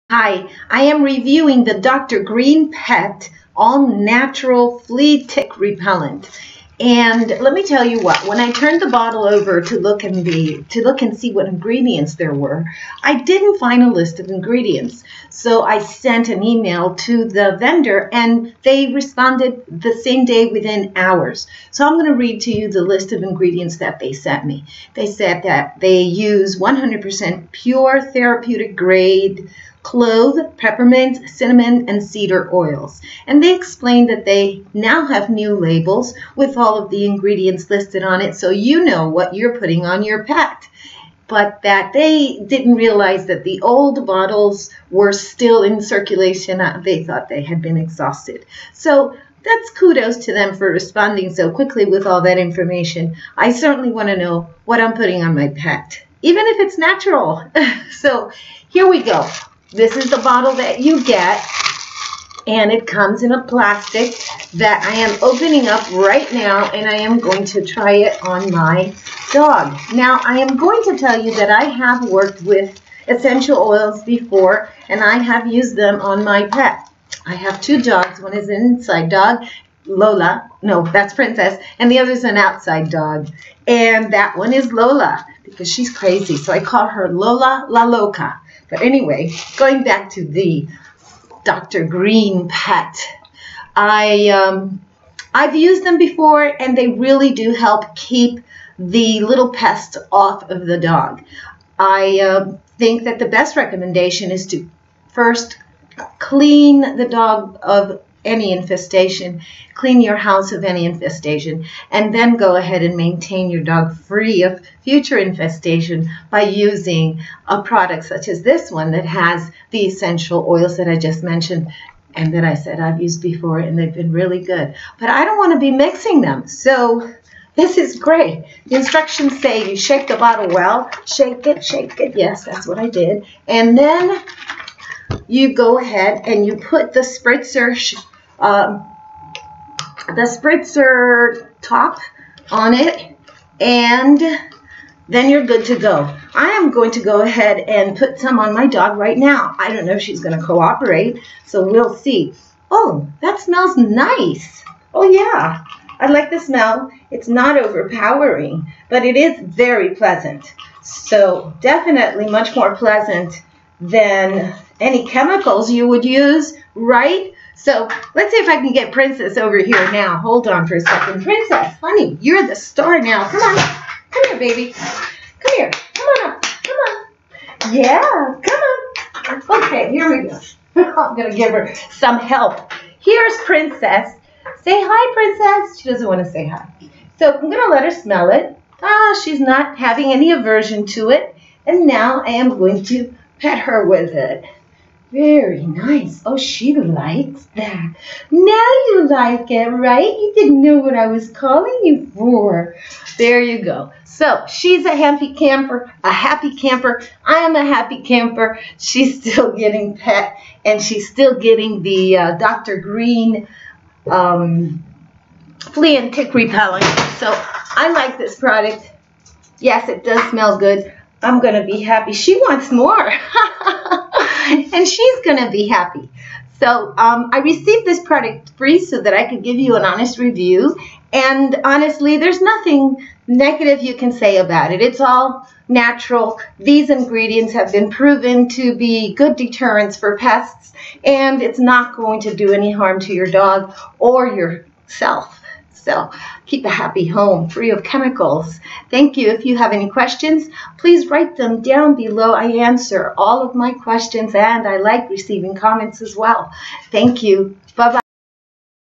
Natural Flea and Tick repellent
This channel and video is not for children. Content for adults only.This is a natural blend of essential oils that is safe to use on your pets and in your home.  100% pure therapeutic grade clove, peppermint, cinnamon, and cedar oils.